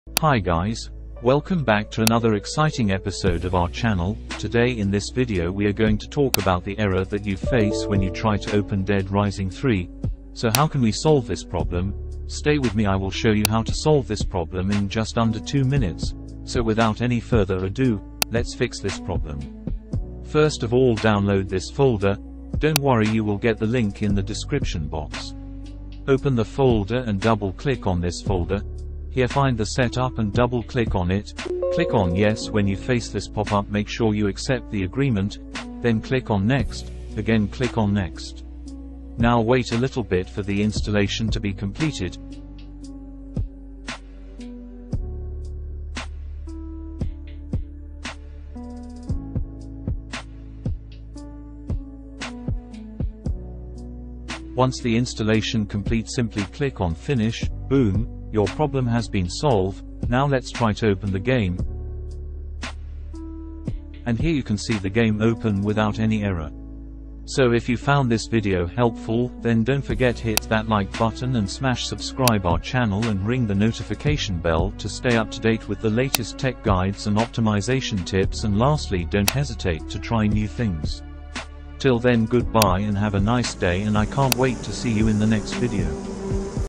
The code execution cannot process because X3DAudio1_7.dll was not found | Dead Rising 3
## The code execution cannot process because X3DAudio1_7.dll was not found. Reinstalling the program may fix this problem

## Description

**Facing the frustrating "X3DAudio1_7.dll was not found" error? Don't worry, you're not alone! This error prevents your programs from launching, especially games. But fear not, there's a fix!**

This video explains what the X3DAudio1_7.dll error means and provides two simple solutions to get you back on track:

1. **Reinstall the Program:** Sometimes, a simple reinstall can replace missing files. The video will guide you through the process.
2. **Update DirectX:** X3DAudio1_7.dll is part of DirectX, a suite of multimedia technologies for Windows. Updating DirectX can often resolve missing dll issues. The video will show you how to update DirectX safely.

**Bonus Tip:** The video also clarifies the difference between 32-bit and 64-bit systems and how that affects dll placement (system32 vs syswow64).

**By following these steps, you should be able to fix the X3DAudio1_7.dll error and get your programs running smoothly again.